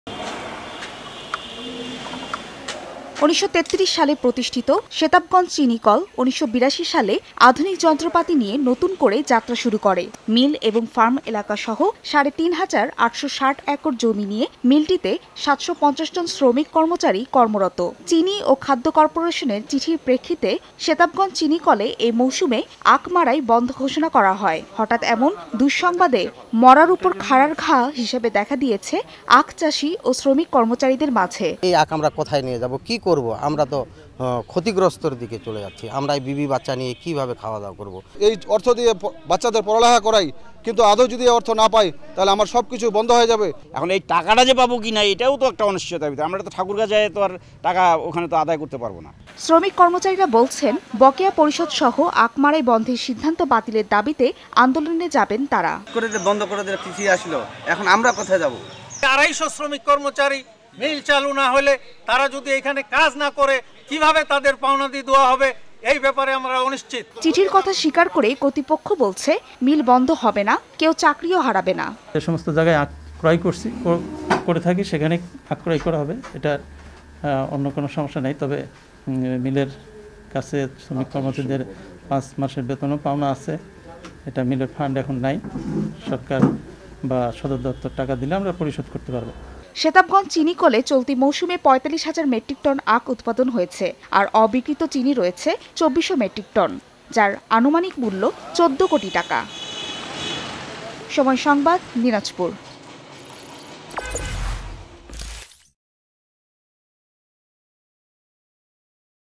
'মড়ার উপর খাঁড়ার ঘা' | Setabganj Sugar Mill | Dinajpur News | Somoy TV
১৯৩৩ সালে প্রতিষ্ঠিত সেতাবগঞ্জ চিনিকল ১৯৮২ সালে আধুনিক যন্ত্রপাতি নিয়ে নতুন করে যাত্রা শুরু করে। মিল এবং ফার্ম এলাকাসহ সাড়ে ৩ হাজার ৮৬০ একর জমি নিয়ে মিলটিতে ৭ শ ৫০ জন শ্রমিক- কর্মচারী কর্মরত। চিনি ও খাদ্য কর্পোরেশনের চিঠির প্রক্ষিতে সেতাবগঞ্জ চিনিকলে এ মৌসুমে আখ মাড়াই বন্ধ ঘোষণা করা হয়। হঠাৎ এমন দুঃসংবাদ